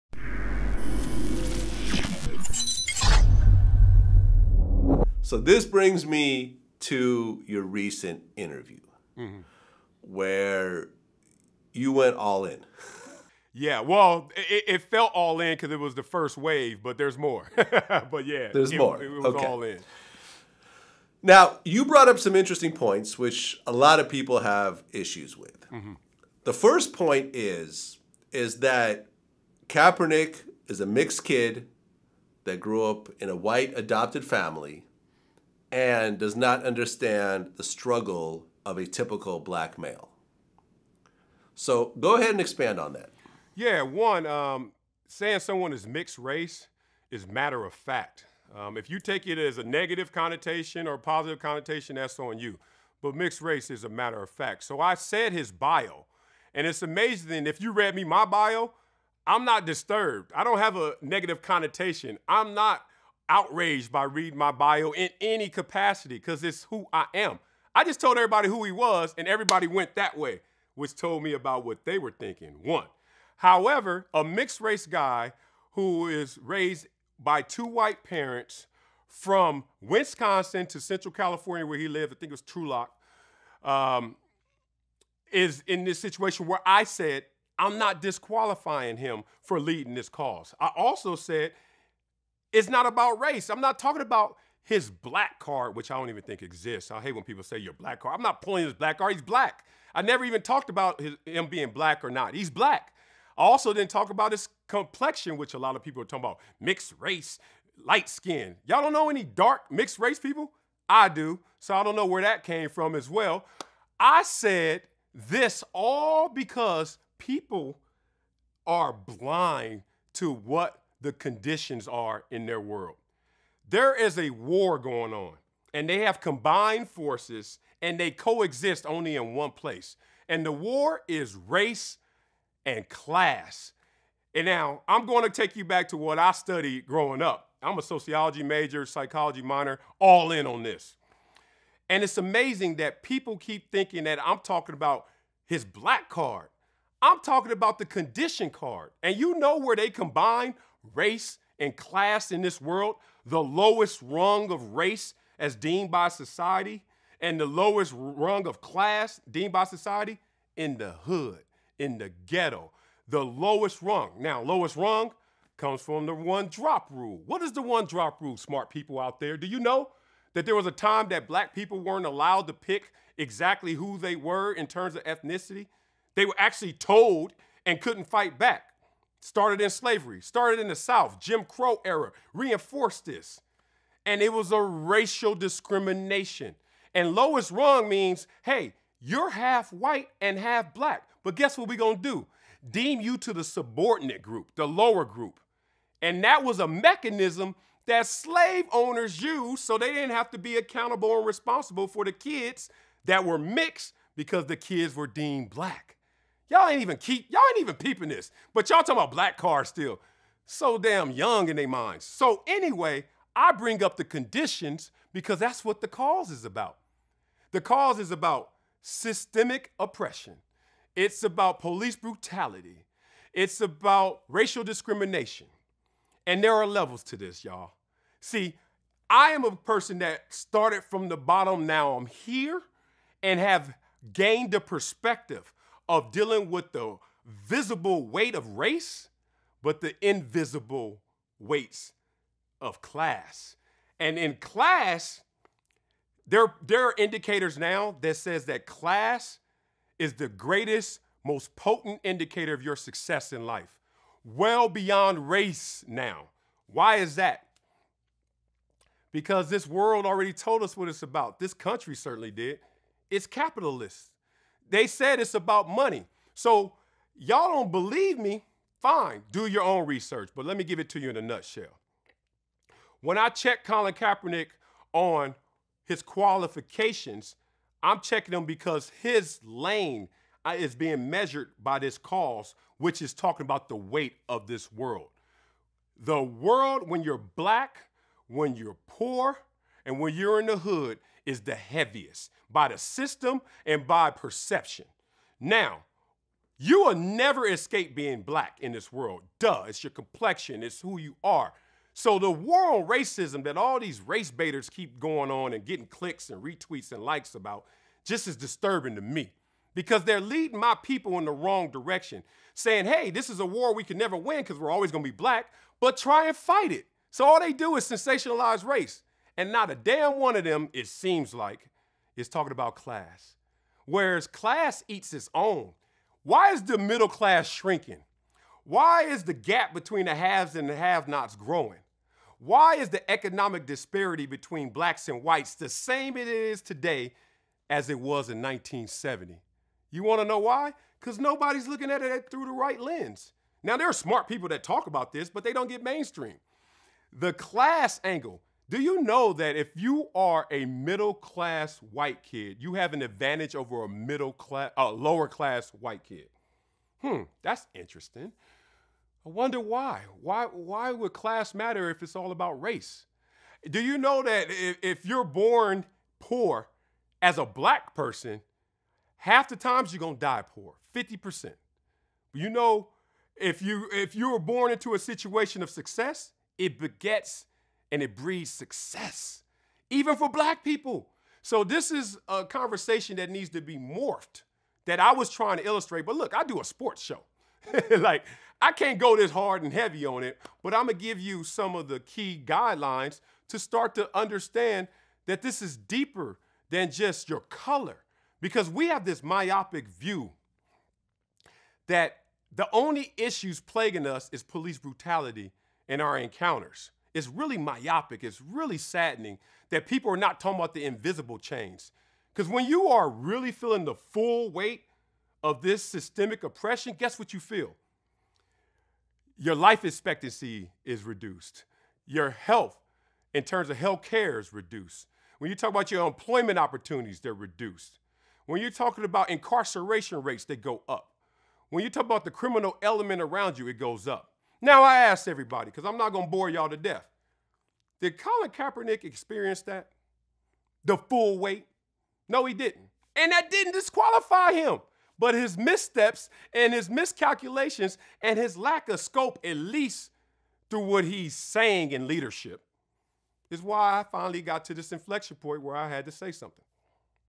Marcellus Wiley on Pointing Out Kaepernick is Mixed with White Parents (Part 3)
Marcellus Wiley opened up about Colin Kaepernick leading a cause for systemic oppression and police brutality, and he questioned whether Kaepernick has felt the weight of systemic oppression. Marcellus explained that when he previously spoke about Kaepernick being mixed race, he wasn't "pulling his black card." He went on to speak about statistics of people growing up in poor neighborhoods, which you can hear more about above.